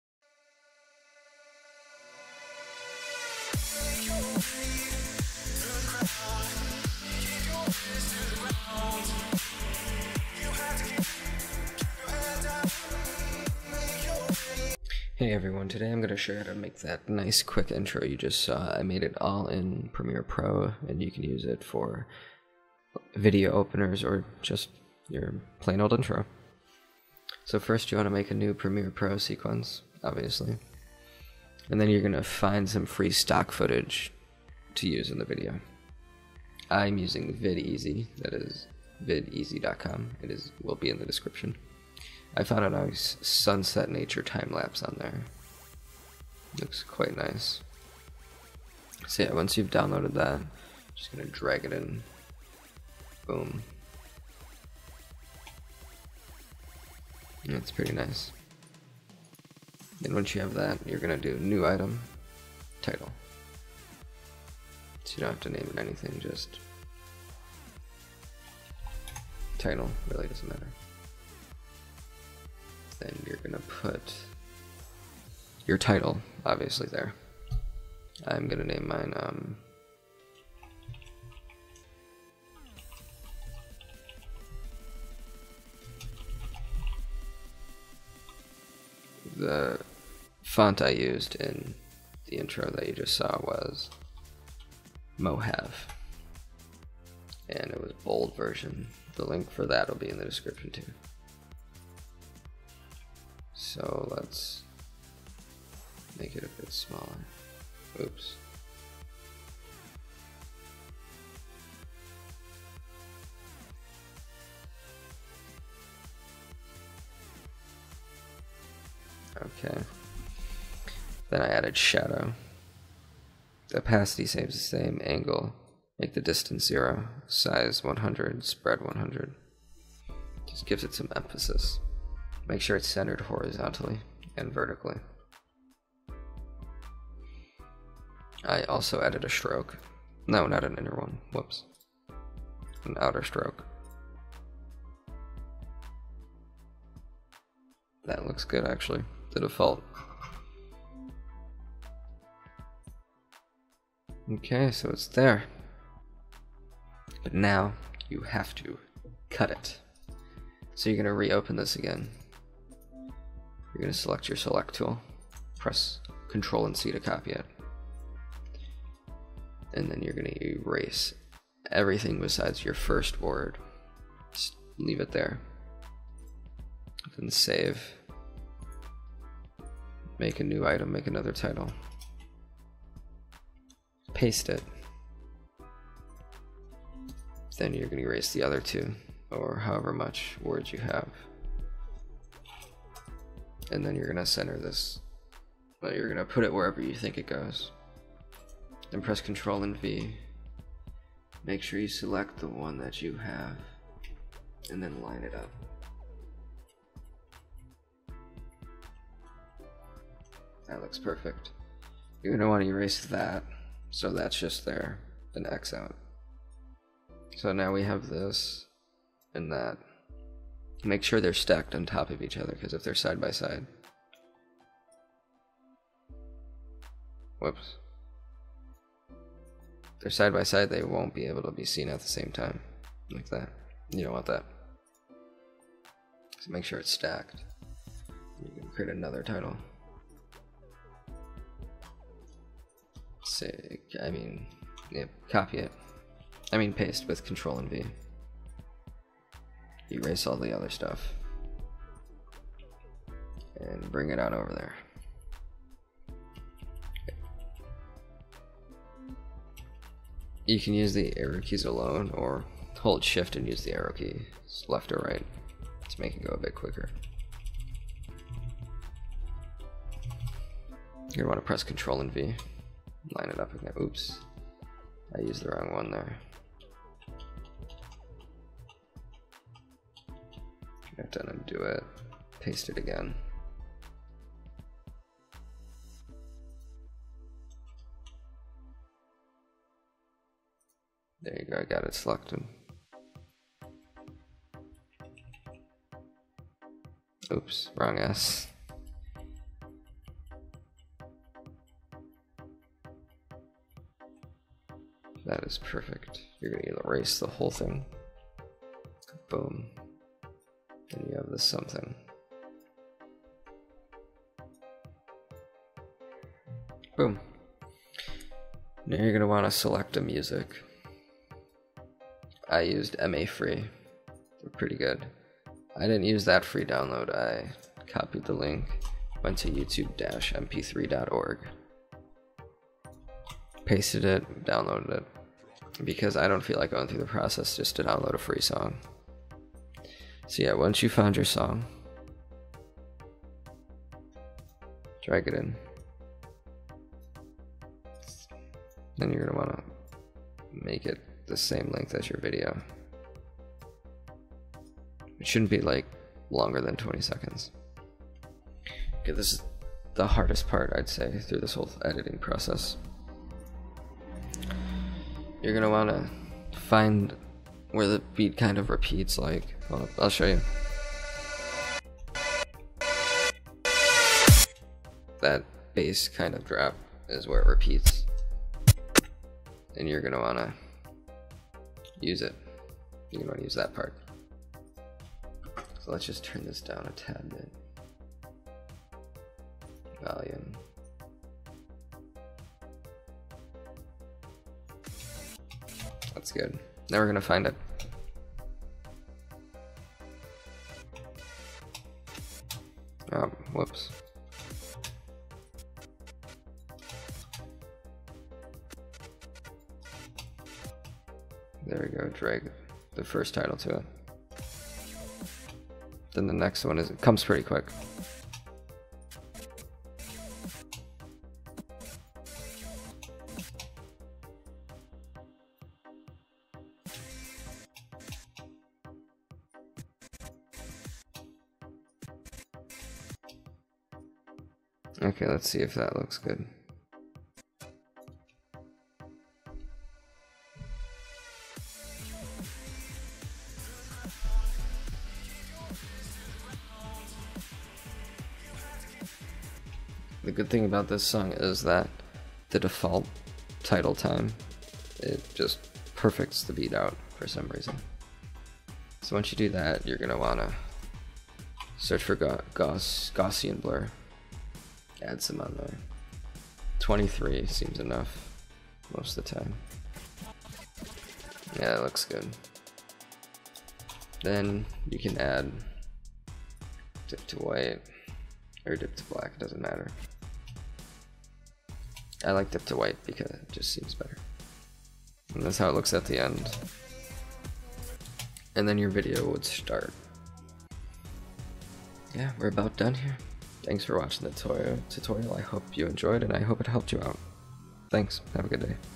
How-to: Make an Intro in Premiere Pro CC
Hello everyone, in this video I will show you how to create a simple video starter/intro for your videos.

➜ Music